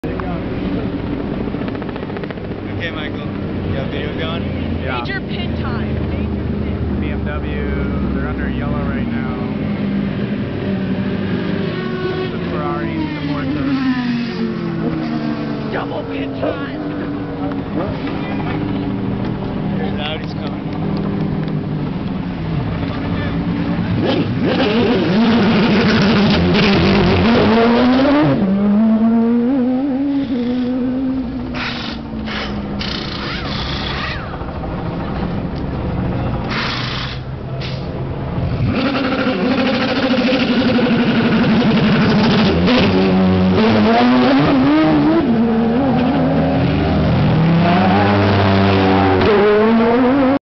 2007 ALMS @ Lime Rock Park - Penske Porsches Pitting
7/7/07 - The 2007 American Le Mans Series New England Grand Prix @ Lime Rock Park, Connecticut.

With Audi VIP passes, Timo, Bex, and I (Michael) got access to Audi's pit area during the race to be up close and personal during a few pit stops of Audi's R10 TDI's.

This video just features the two Penske Motorsports Porsche RS Spyders (LMP2) pitting under a yellow.